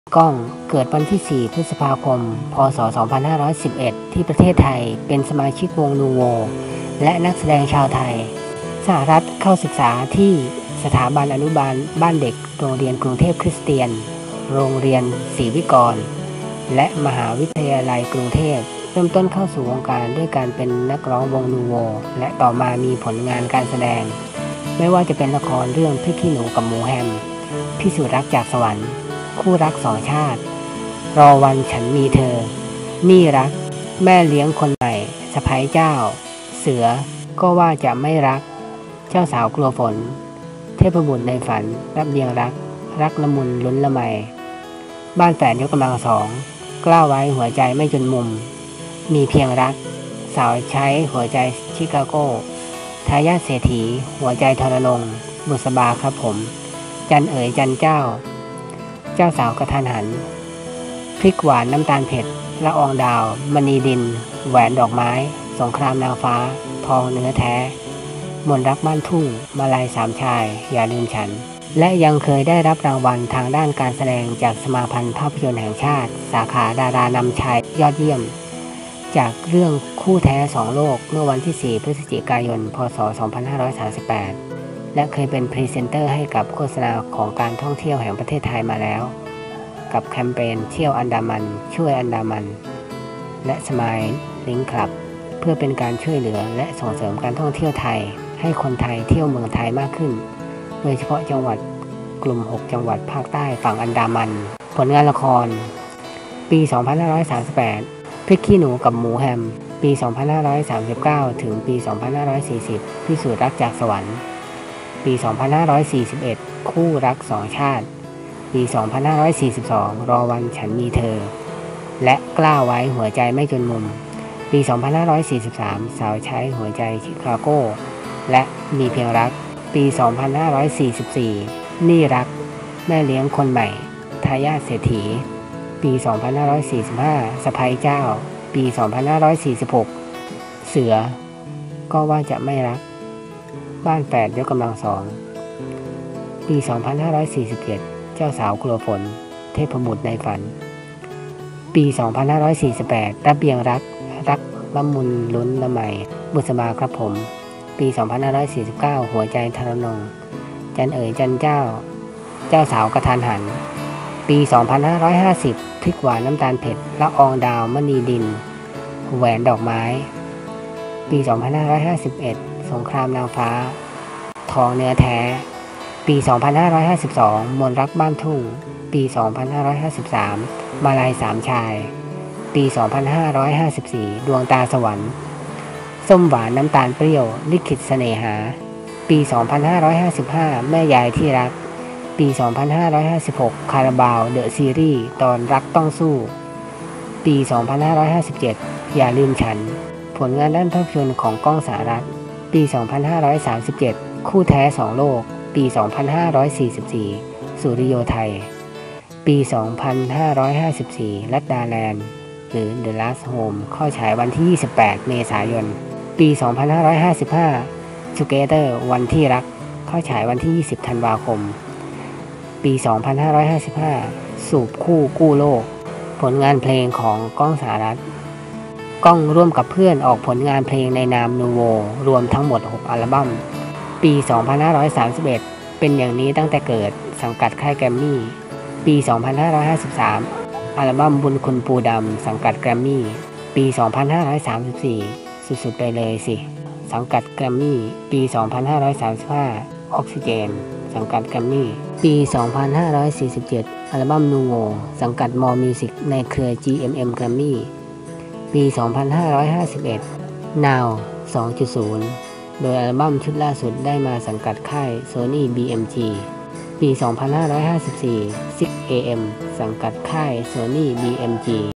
ประวัติ ก้อง สหรัถ สังคปรีชา
ขอบคุณภาพข่าวจาก : Google.com

สหรัถ สังคปรีชา เกิดวันที่ 4 พฤษภาคม พ.ศ. 2511 ที่ประเทศไทย เป็นสมาชิกวงนูโว และนักแสดงชาวไทย วิกิพีเดีย
เกิดเมื่อ: 4 พฤษภาคม 2511 (อายุ 47), สหรัฐอเมริกา
การศึกษา: มหาวิทยาลัยกรุงเทพ
พี่น้อง: ฐากูร สังคปรีชา
ภาพยนตร์และรายการโทรทัศน์: ลัดดาแลนด์, วงษ์คำเหลา เดอะซีรีส์, วันที่รัก
รางวัลที่ได้รับการเสนอชื่อเข้าชิง: รางวัลภาพยนตร์แห่งชาติ สุพรรณหงส์ สาขาผู้แสดงนำชายยอดเยี่ยม